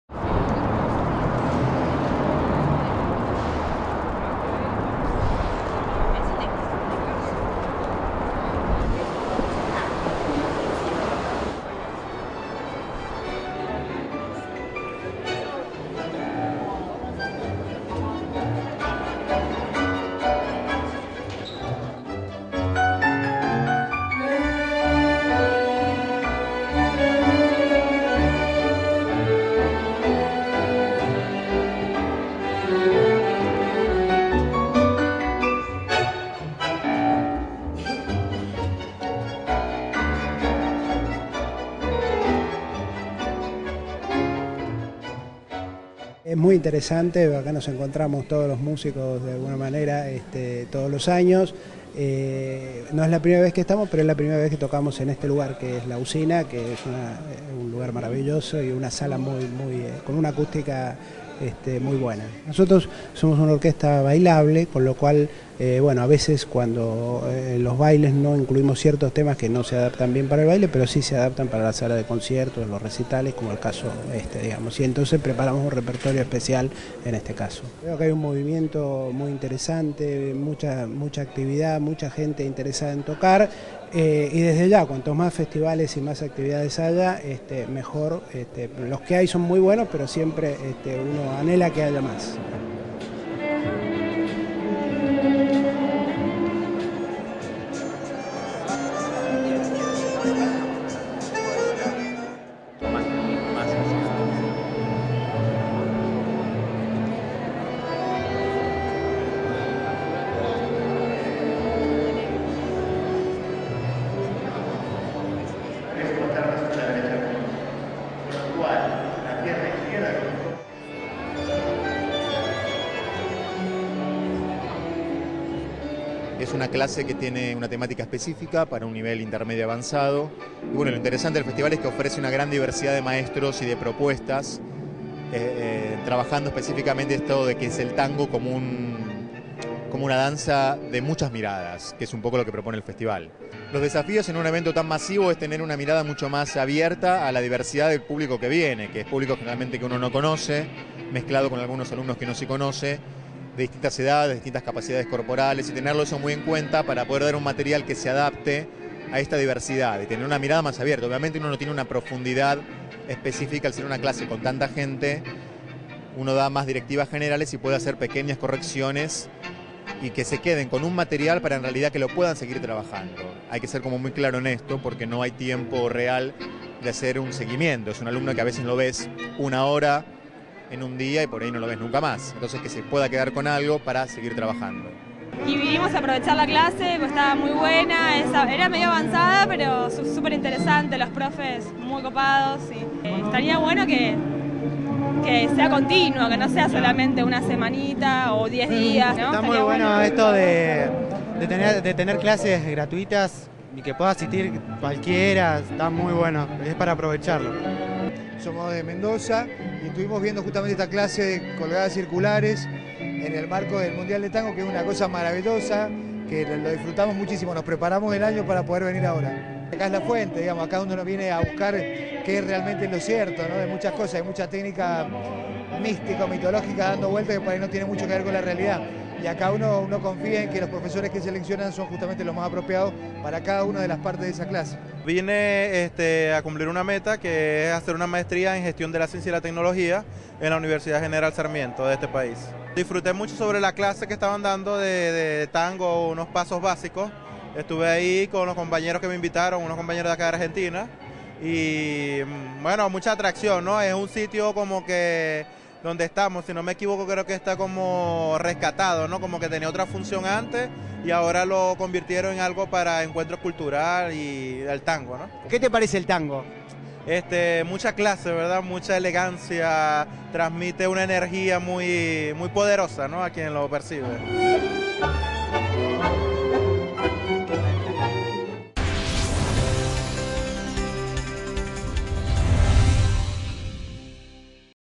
Conciertos y milongas populares llenaron de música y color el Festival de Tango
En su último fin de semana, diversos conciertos y milongas populares colmaron de música y color todas las sedes en donde se llevaron a cabo las actividades que formaron parte del Festival Mundial de Tango, que se celebra desde 14 de este mes en la Ciudad de Buenos Aires.

En un domingo que comenzó con un concierto del gran director indio Zubin Metha y la Filarmónica de Israel, frente al Puente Alsina, tanto el Centro de Exposiciones como la Usina de las Artes albergaron a miles de personas que se acercaron para escuchar buena música y disfrutar de unos pasos de baile.

Las clases libres y gratuitas de baile fueron una de las principales atracciones del Festival, con la presencia de parejas de distintos lugares el país y turistas extranjeros deseosos de aprender los pasos básicos.

También se llevaron a cabo shows de distintas agrupaciones típicas y milongas populares, a modo de cierre del encuentro.